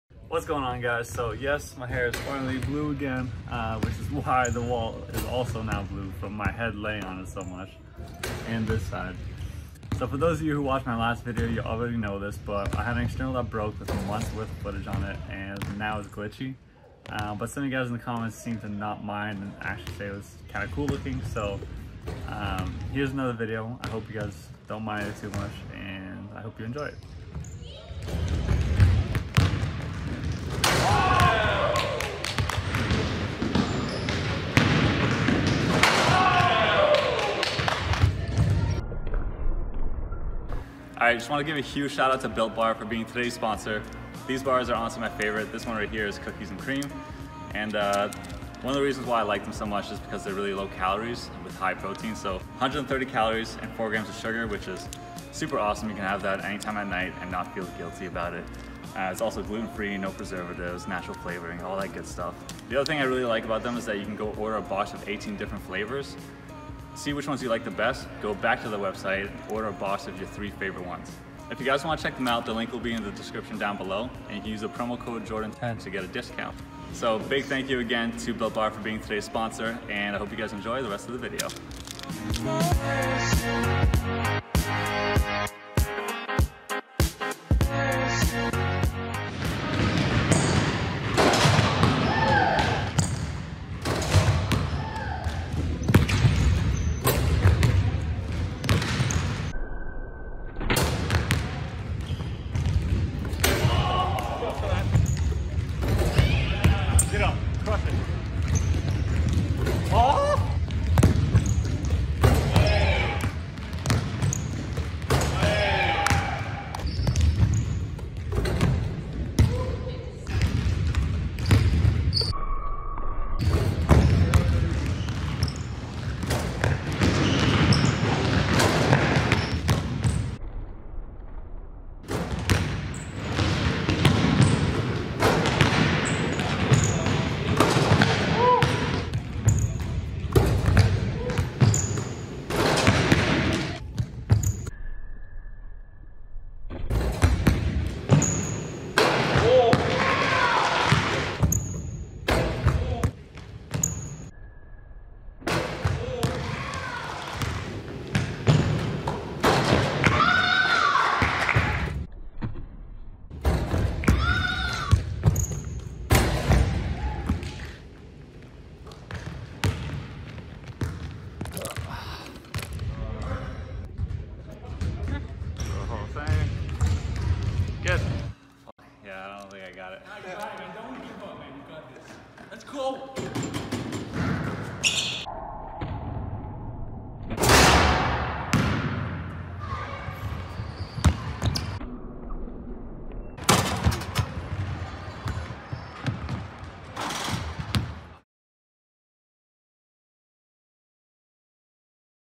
Jordan Kilganon Does the Scorpion Dunk!!
Click the link to check out all of BuiltBar's flavors!

Shout out to Mike Lamy for the gym access!

Other Dunkers: Hayden, Jake, Trekk